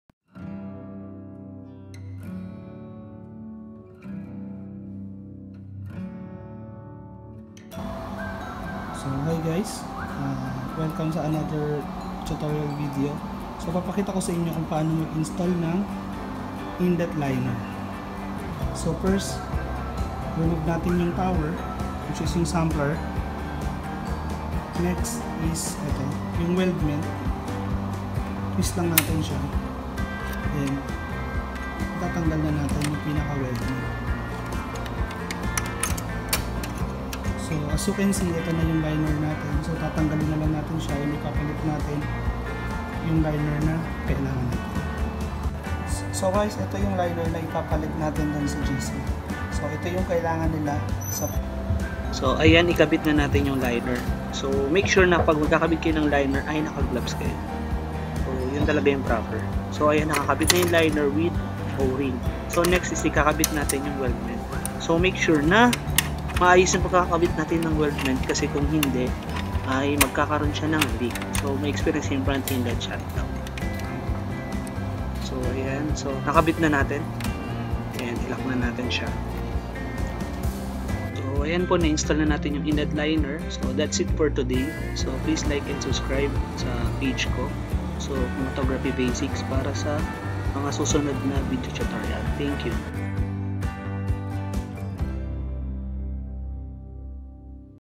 how to install inlet liner - philippines
This video shows how go install inlet lined for your gas chrom